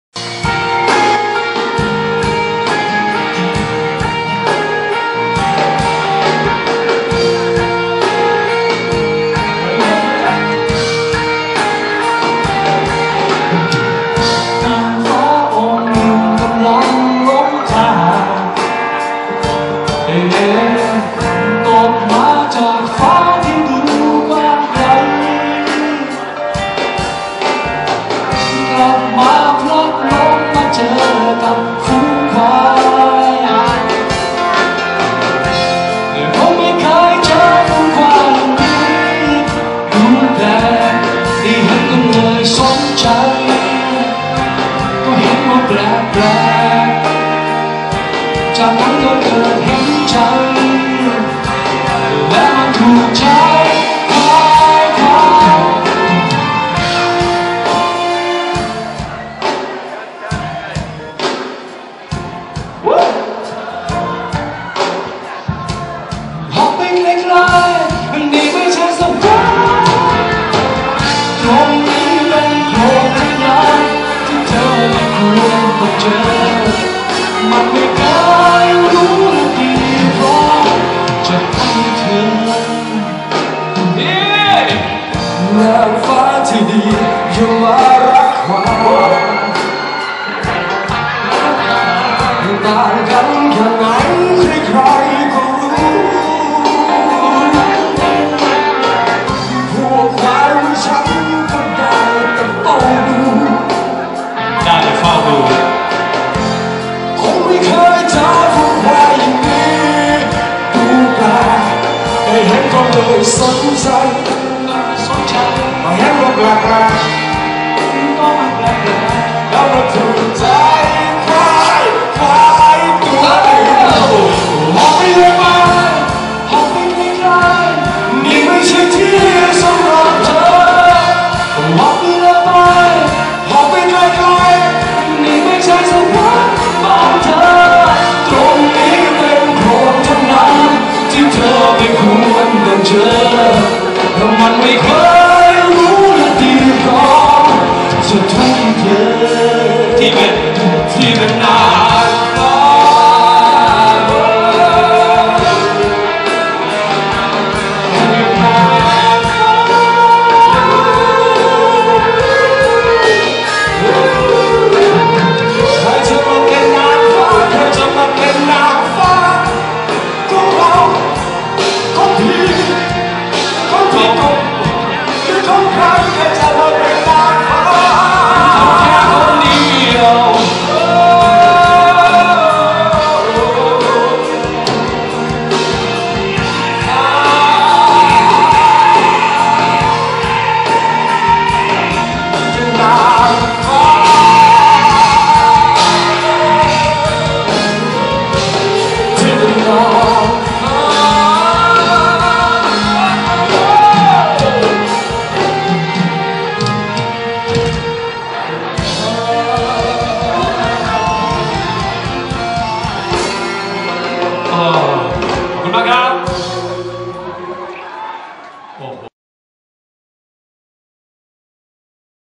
พีสะเดิด แจม กับ กบ แท็กซี่ ... มันส์สุดมาก
ลีลาทั้งสองท่านสุดยอดมากครับ ผมกลัวตอนพี่กบ เเท็กซี่โยนไมค์หาคนดู กลัวไมค์หลุดไปใส่หัวคนจัง 555 มันส์ที่สู๊ดดดด